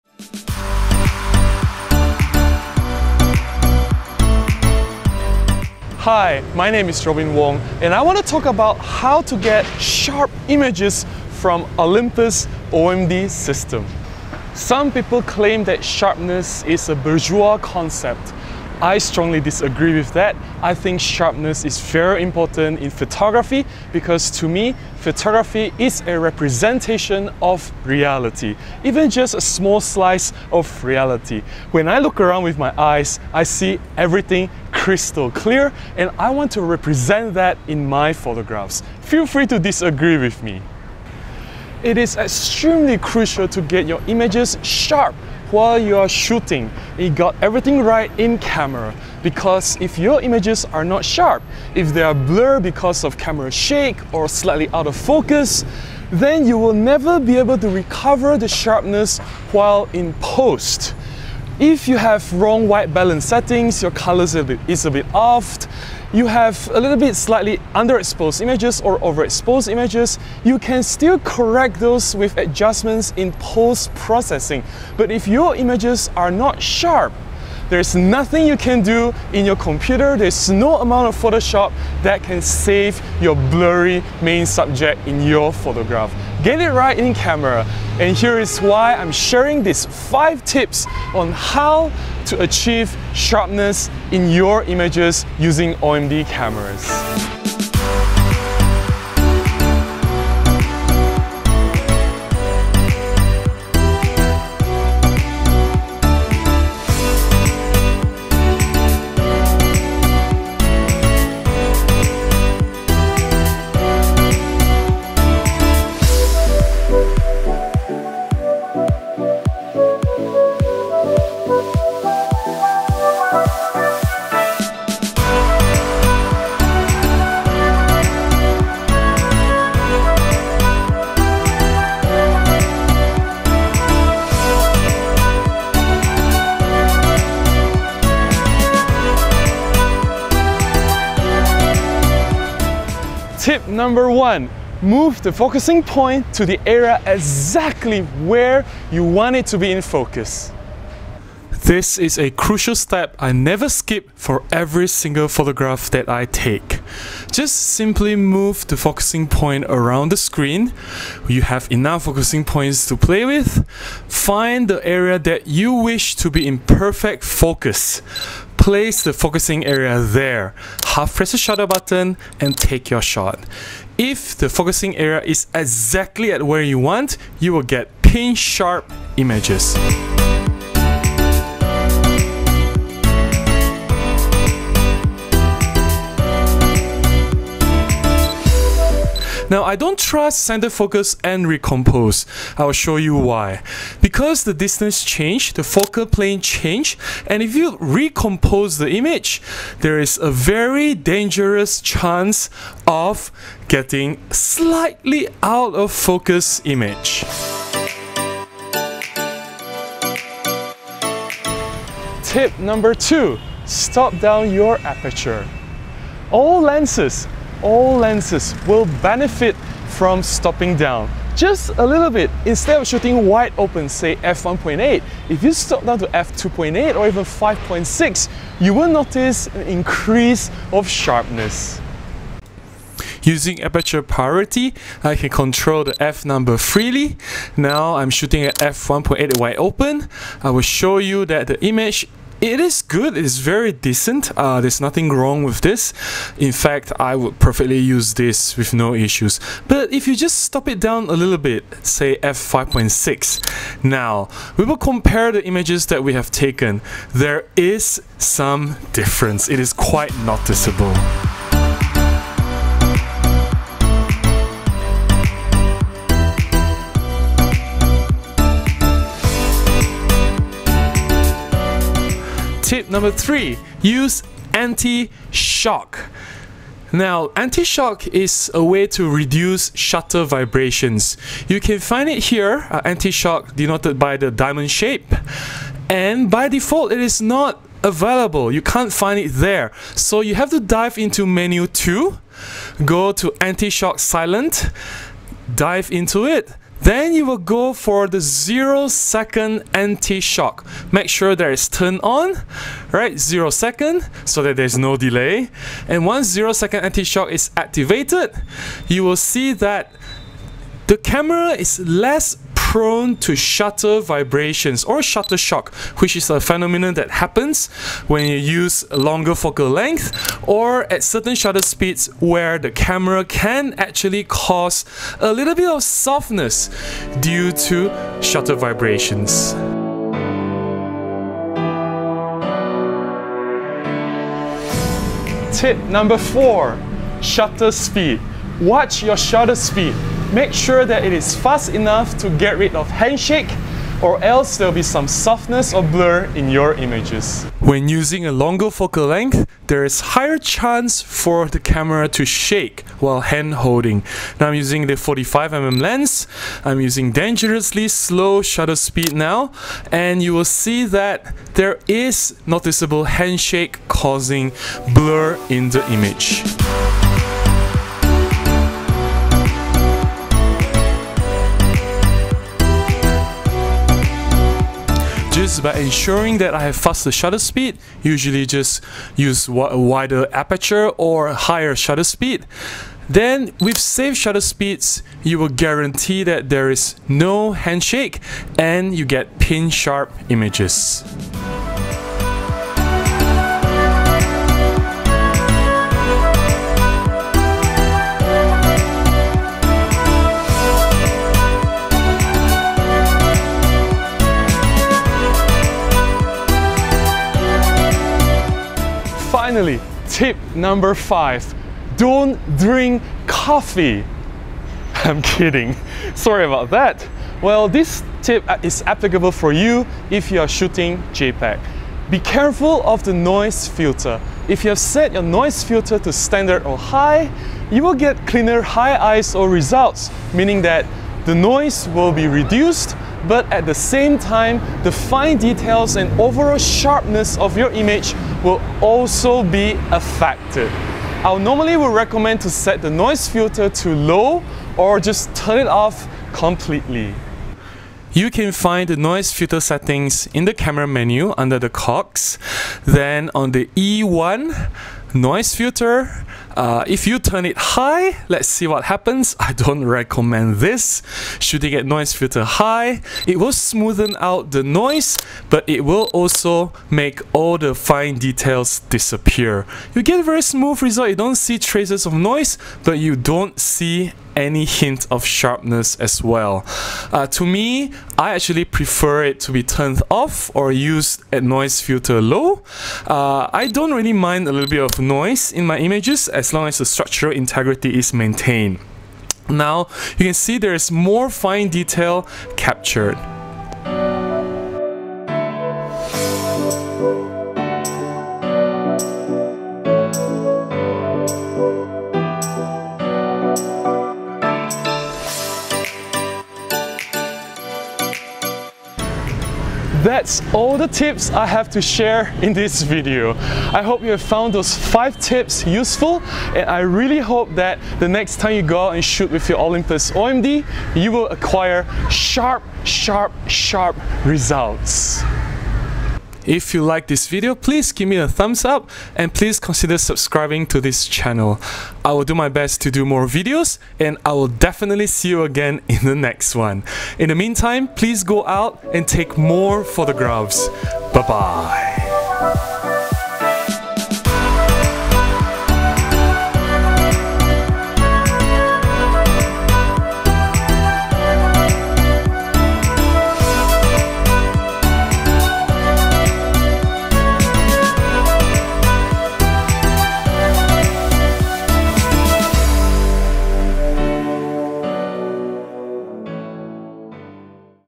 How to Get Sharp Images With Olympus OM-D
Sharing some crucial tips that can help optimize sharpness in images from your Olympus OM-D camera. Olympus OM-D E-M1 Mark II
I am an Olympus Visionary and professional photographer based in Kuala Lumpur, Malaysia. This video is made in accompaniment to a blog article, link here:

One of the more popular questions thrown at me by my blog readers over the years was how I got my images so sharp? There was as if I had some secret technique on achieving tack sharp images being shown on my blog. Honestly, there are no secrets, and I am sharing important steps on how to acquire pin sharp images using Olympus OM-D cameras, which I have been shooting with for years now.